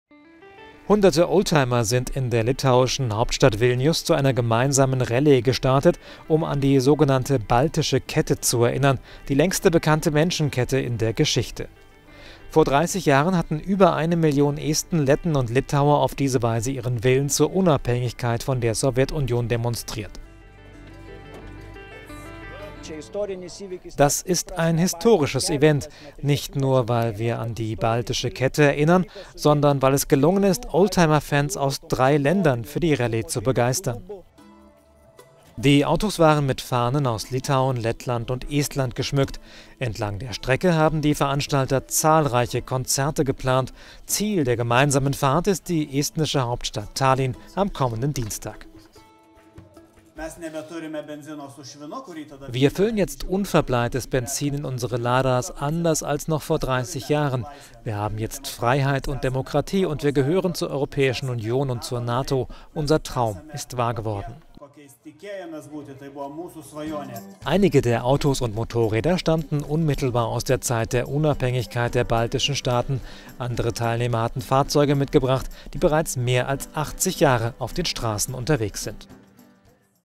Balten erinnern an die längste Menschenkette der Welt
Die sogenannte "Baltische Kette" war die bisher längste Menschenkette der Geschichte. Mit einer Oldtimer-Rallye haben die Menschen in Litauen heute daran erinnert.…